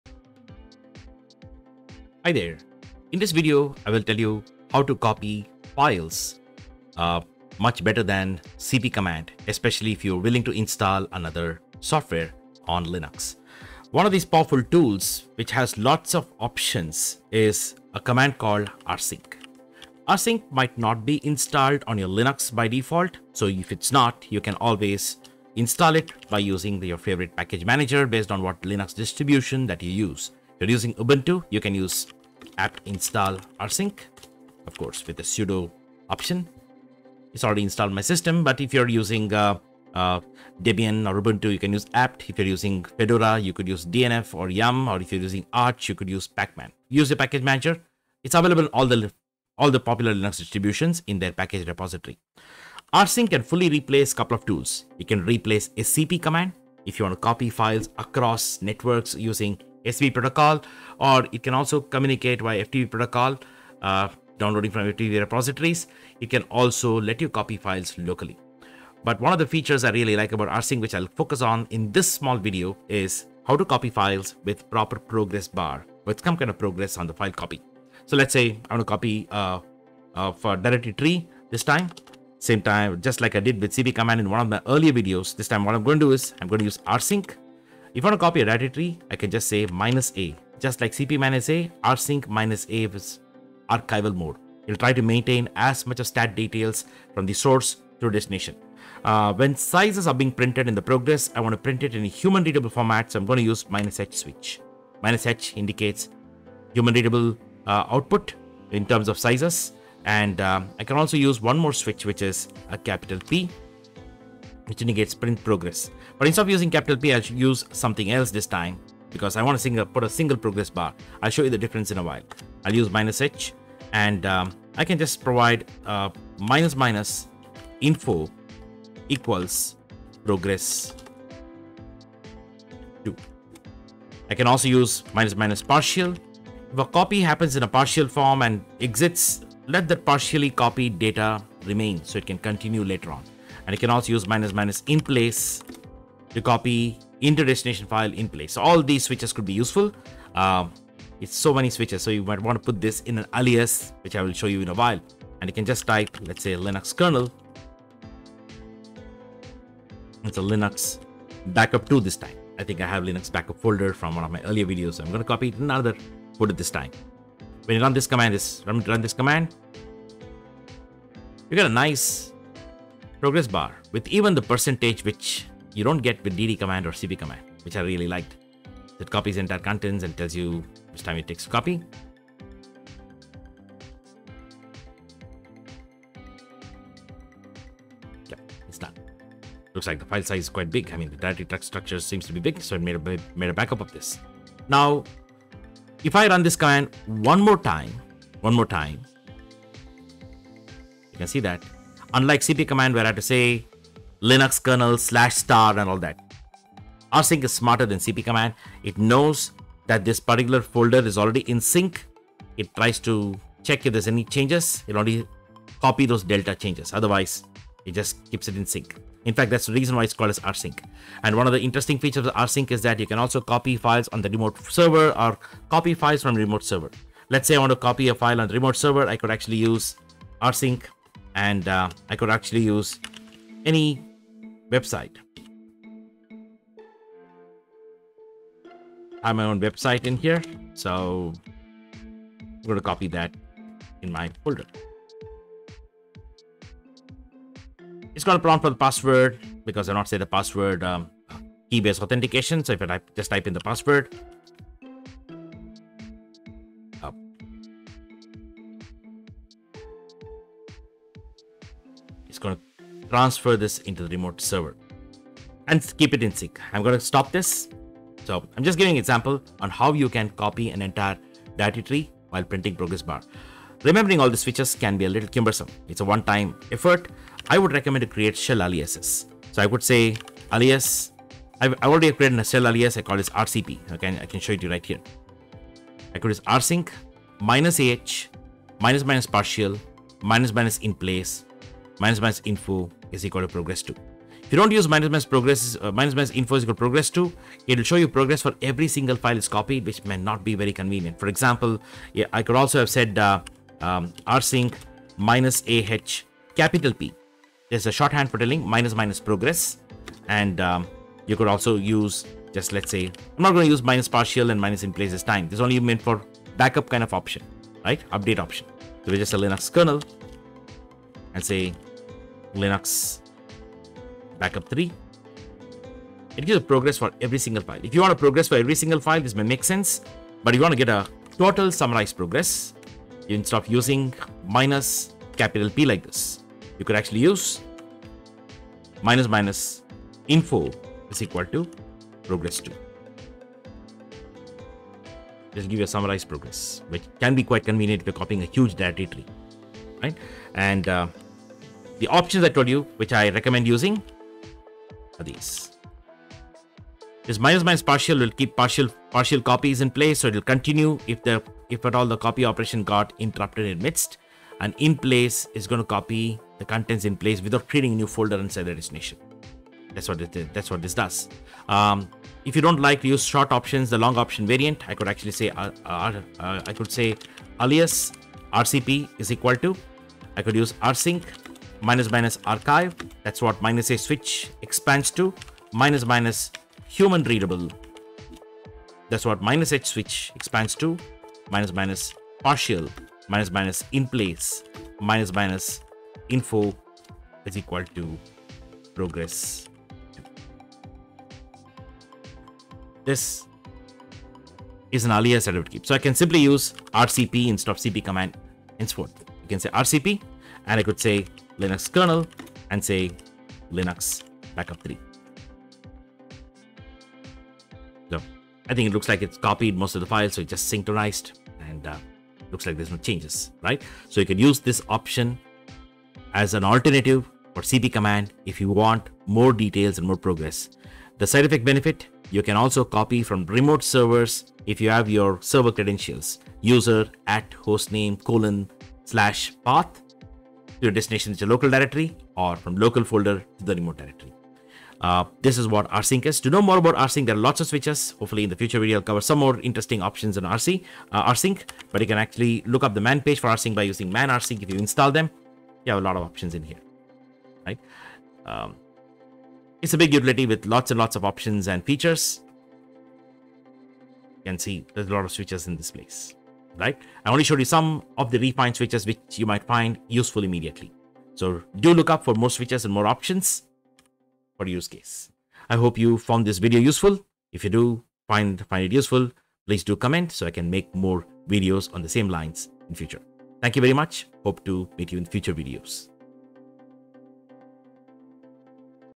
Linux command-line: Copy files recursively with progress information using the 'rsync' utility
The 'rsync' utility is a powerful tool that lets you mirror directory trees across machines.

This video teaches you how to replace your 'cp' command with 'rsync' instead for copying files while printing the progress of copy operations.